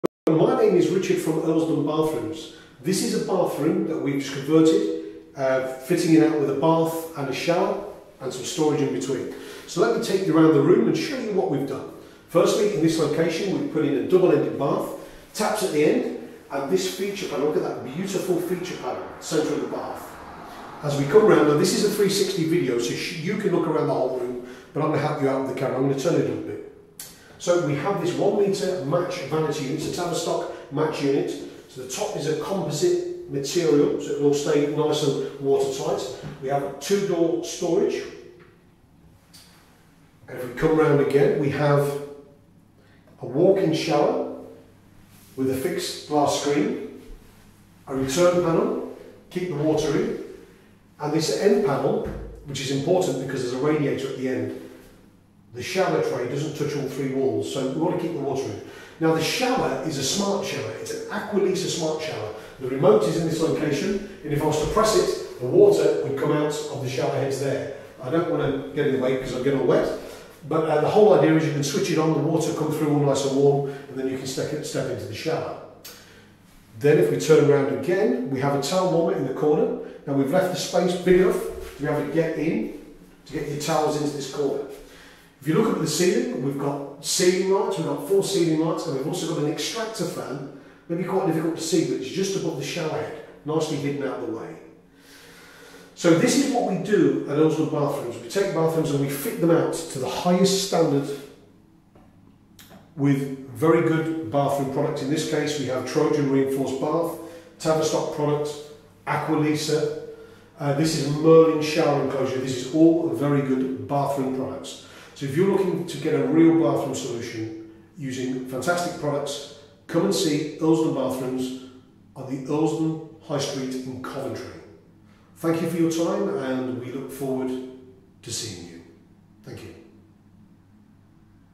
Bathroom Fitted with Walk In Shower and Separate Bath
This is a bathroom renovation that we carried out. The new bathroom design includes both a walk in shower and a separate double ended bath. The shower is an aqualisa smart shower. There is a feature tile panel over the center of the bath.

Earlsdon Bathrooms offer high quality complete bathroom solutions with exemplary workmanship.

Visit our showroom at 28 Earlsdon Street, Coventry, CV5 6EJ.